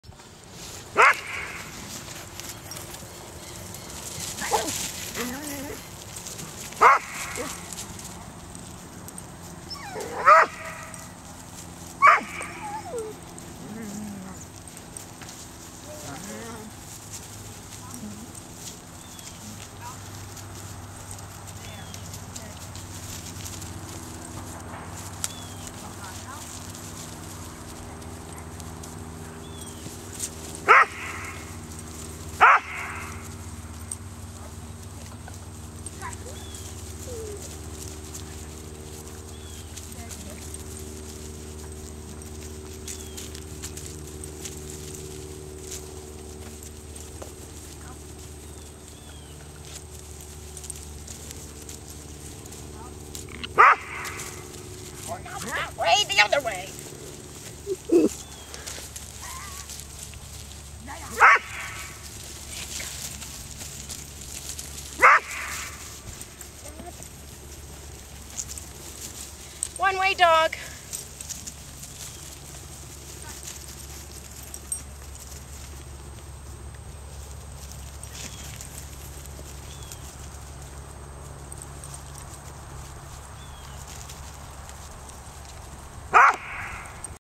Bonnie
this is bonnie, she is a splash/corrie pup.  she is 5 mos old.